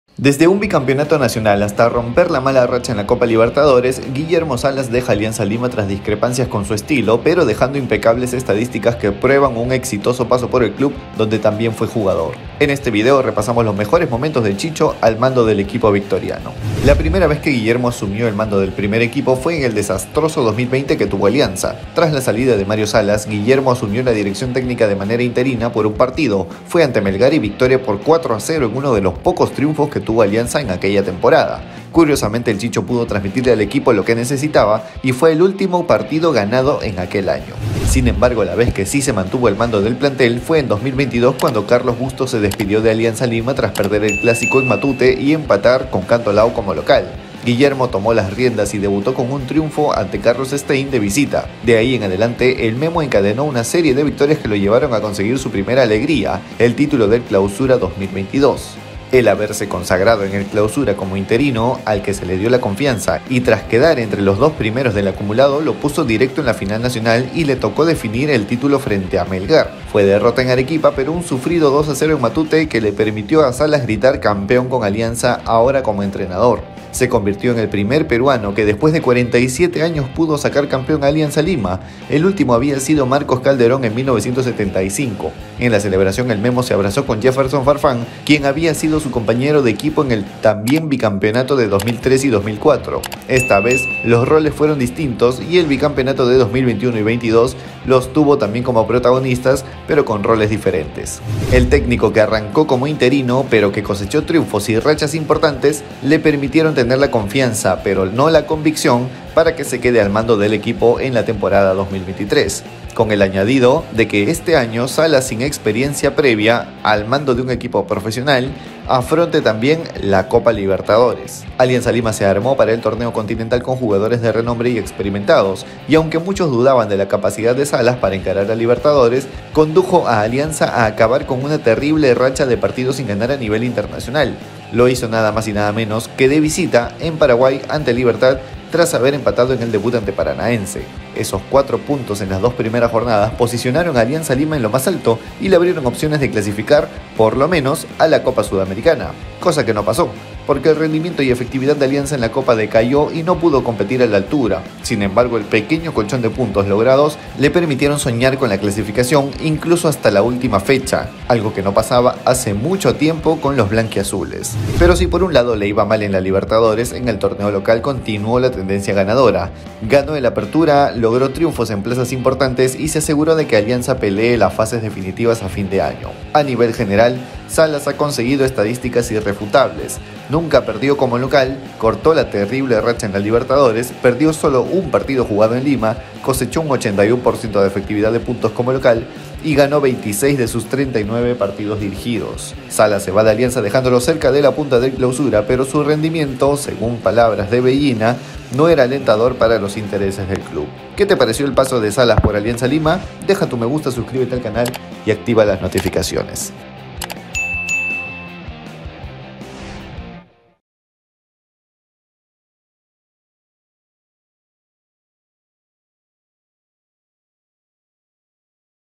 🔵⚪ Las razones por las que los HINCHAS DE ALIANZA LIMA no olvidarán al 'CHICHO' SALAS 🧢
Guillermo 'Chicho' Salas dejó de ser entrenador de Alianza Lima dejando títulos y estadísticas irrefutables. En este video repasamos los más importantes logros del 'Memo' Salas en su paso como entrenador del equipo 'blanquiazul'